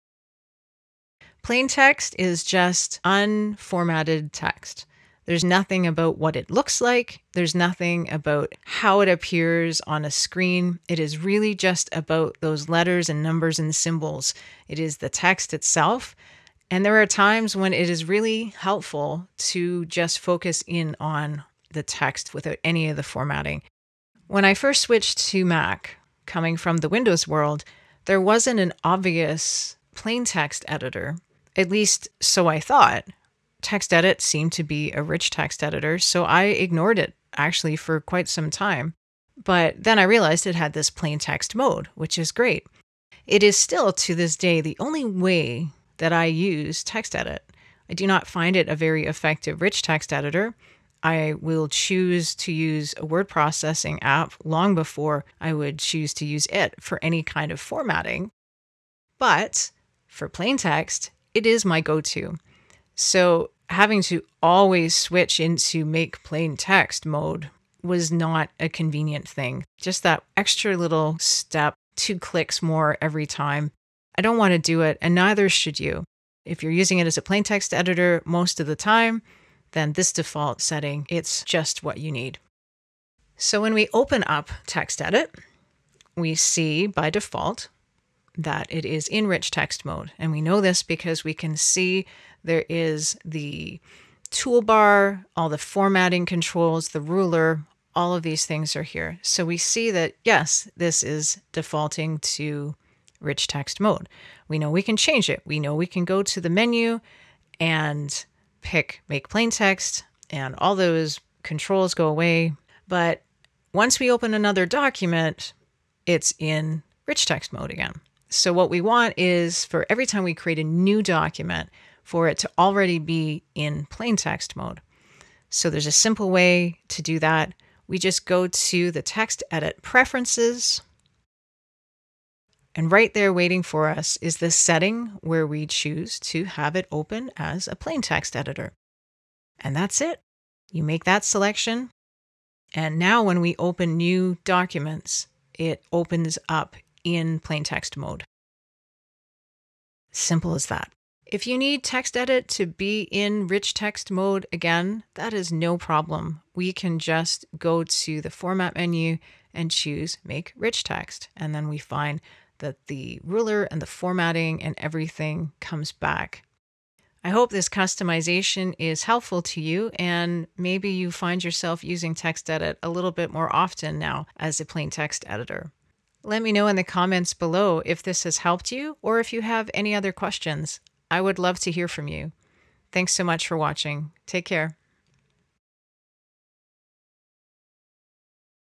How to Make TextEdit Your Default Plain Text Editor for Good
If you use TextEdit as a plain text editor often enough you soon notice how annoying those couple of extra clicks are: Format - Make Plain Text.

Follow along with this week's video and learn how to make plain text mode TextEdit's default setting for good and skip those two extra clicks.